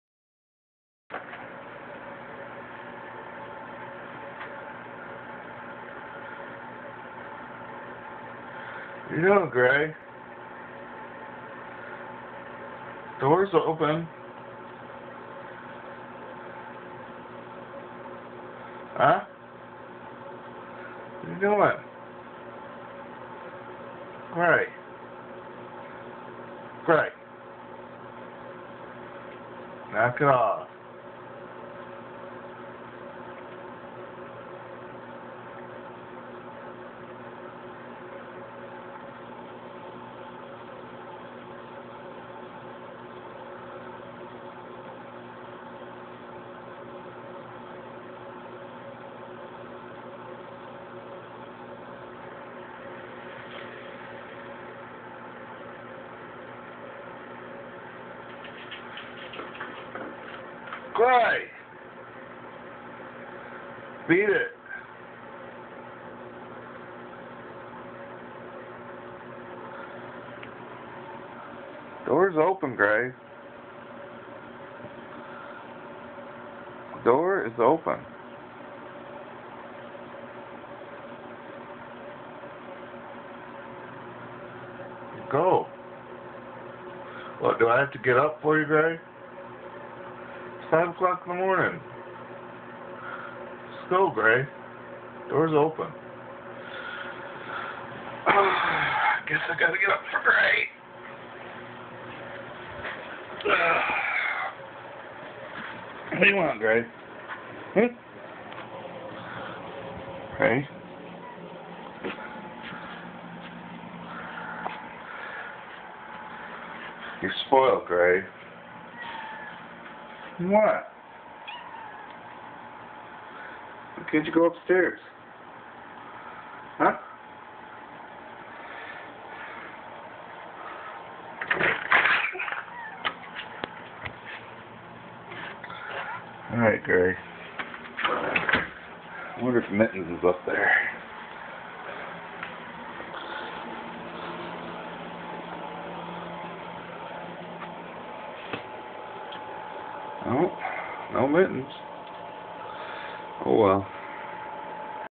How my cat let's me know she wants out at night
Usually between 3am and 5am she will bang on shades until I get up and open door for her.